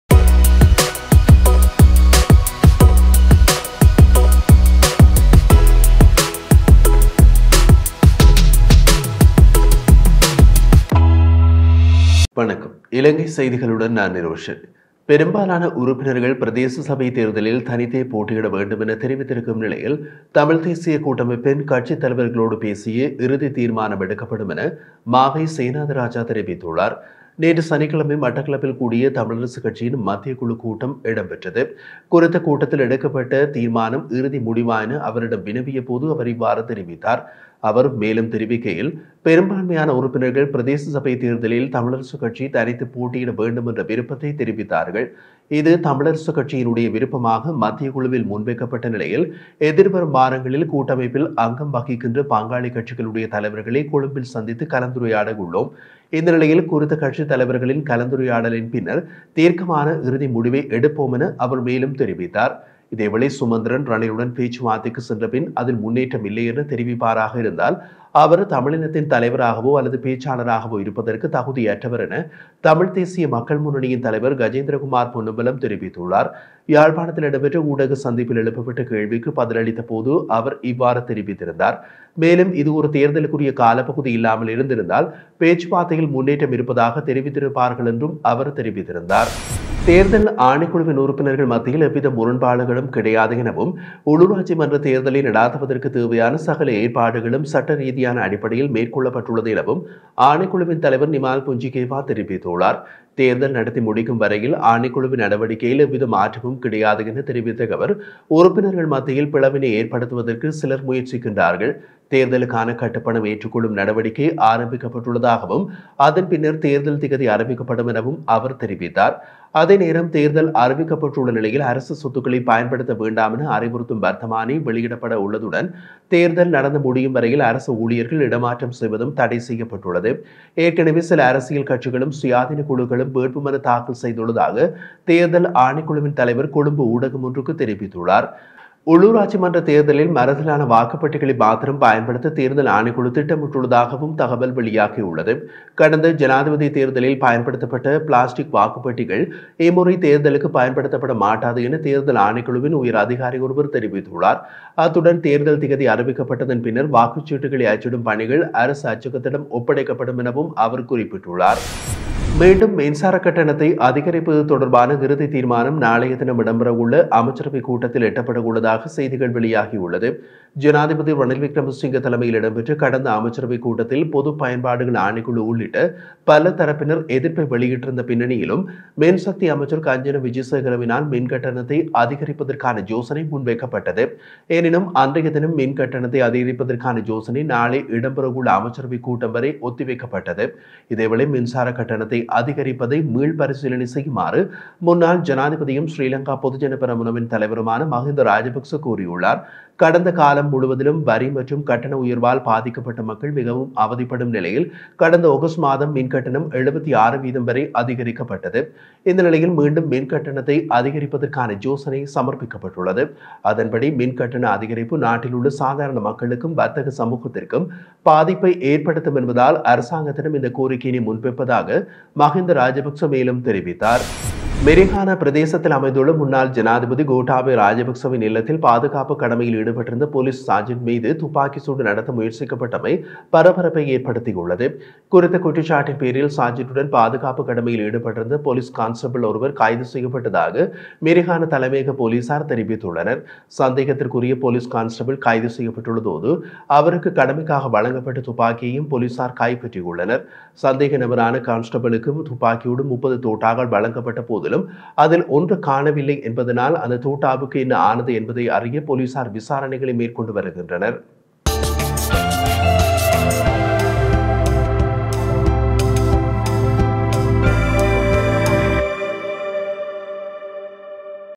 கோட்டாபய வீட்டில் துப்பாக்கிச் சூடு? | Srilankan Tamil News
While most of the members have said that they should contest the Pradeshiya Sabha election alone, the final decision will be taken after talking to the party leaders of the Tamil National Federation, Mawai Senathiraja said.

The Central Committee meeting of the Tamil Nadu Party in Batticaloa was held yesterday on Saturday. He said this when he was asked whether the decision taken in the said meeting was final.

Show : Sri Lanka Tamil News